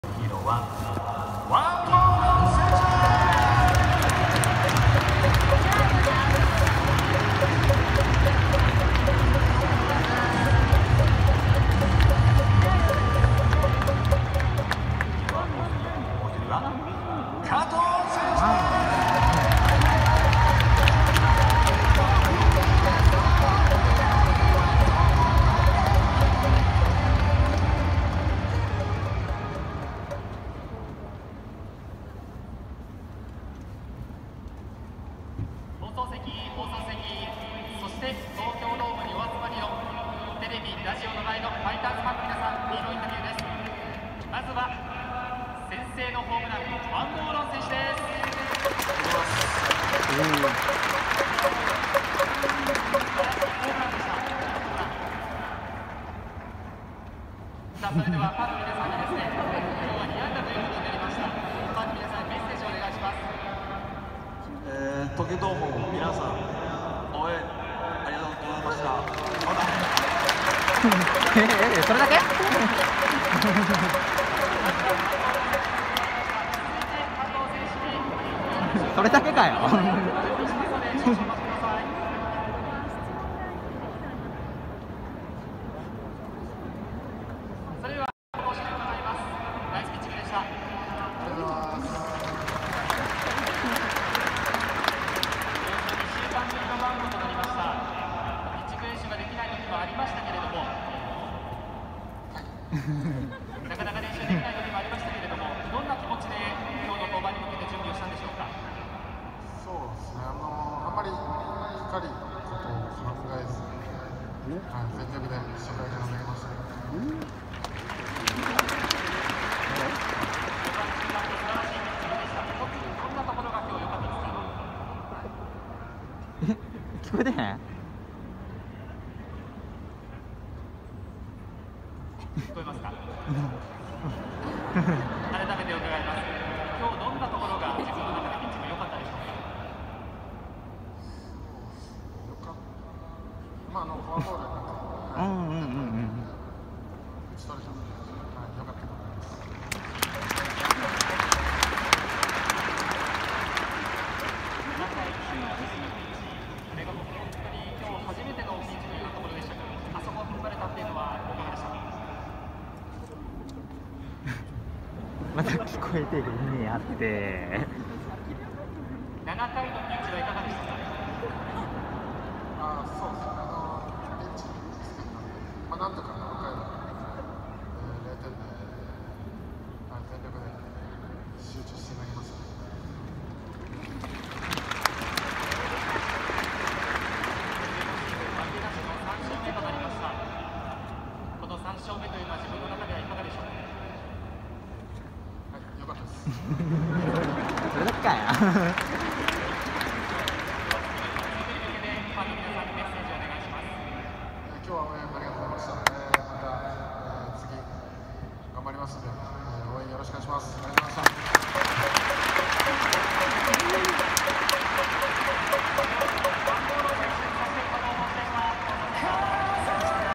2021/5/12 日本ハム 王柏融 加藤貴之 ヒーローインタビュー
2021/5/12 日本ハムvsオリックス 東京ドーム

王柏融は応援歌通り強敵を打ち崩す全壘打を打ちました。